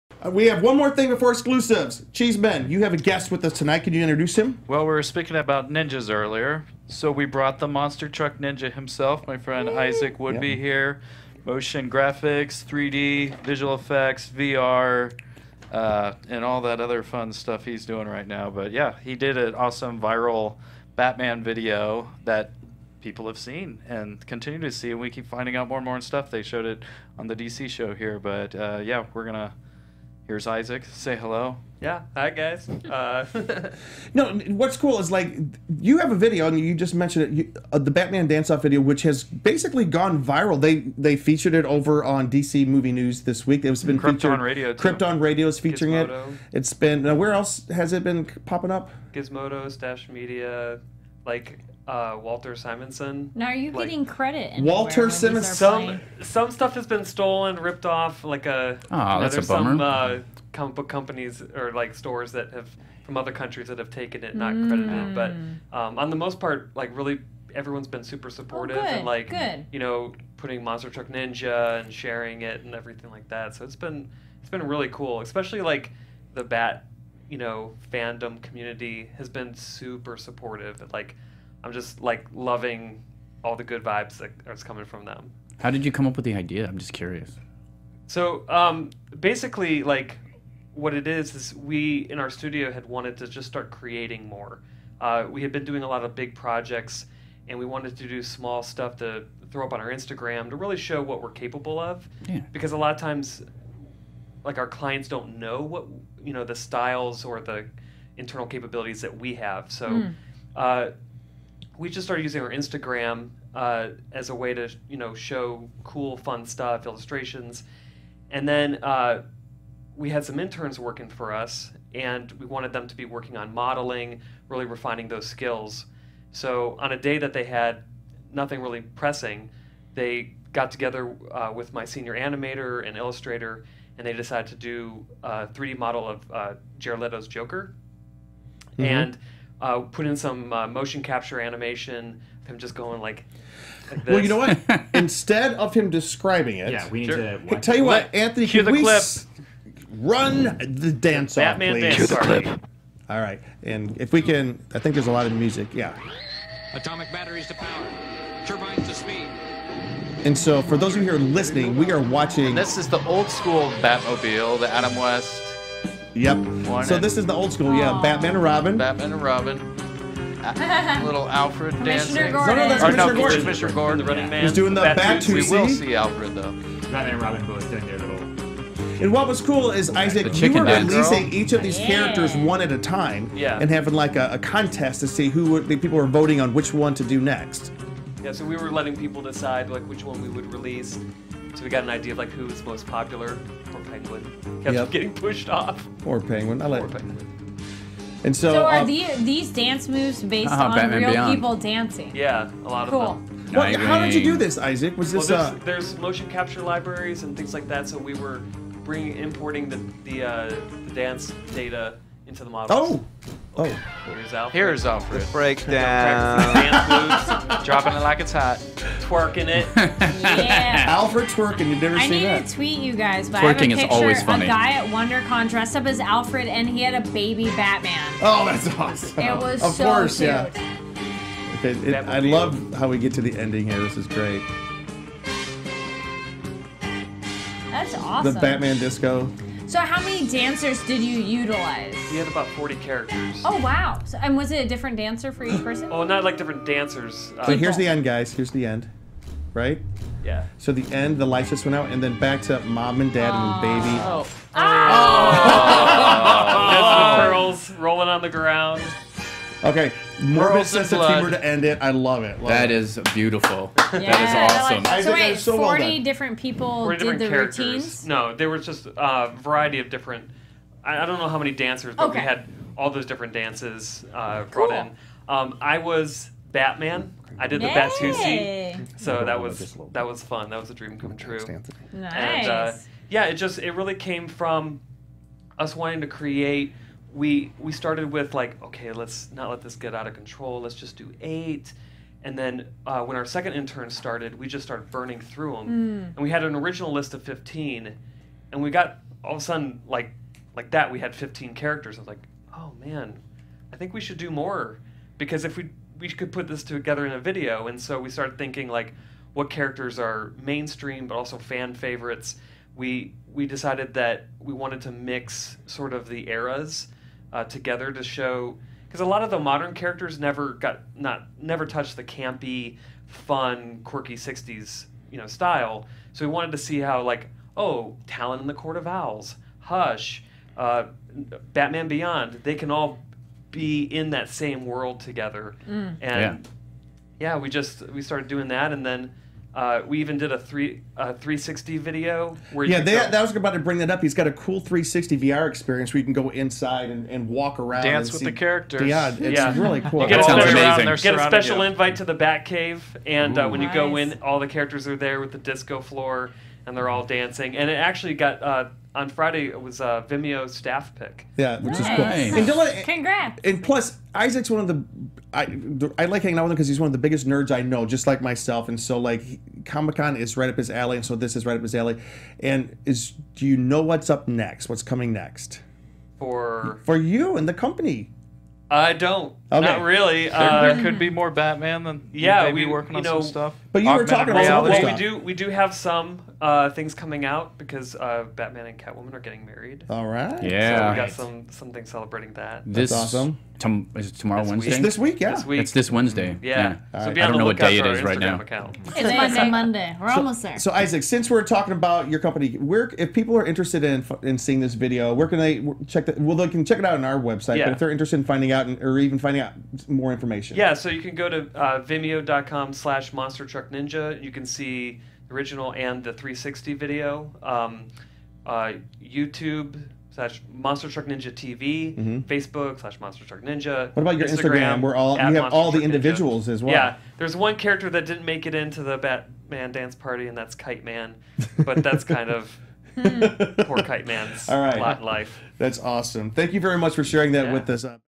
Con Guy Features "Batman Dance Party" & Monster Truck Ninja
Batman Dance Party and Monster Truck Ninja were featured on the Con Guy Podcast!  Owner and exec. creative director, Isaac Woodby, was interviewed about the Batman Dance Party video that's getting love from the comic book community.  Get a behind the scenes look at how the video was inspired and created!

Con Guy Hosts: Jim Frye @conguyjim, Luke Cheeseman @CheeseOnCouch, Kari Land @karidlane, Derek Samm @conguyderek.

GUESTS: Katie Kawamoto @kt_christine, Ron Swallow @dorkyswallow, Isaac Woodby @MonsterTruckNinja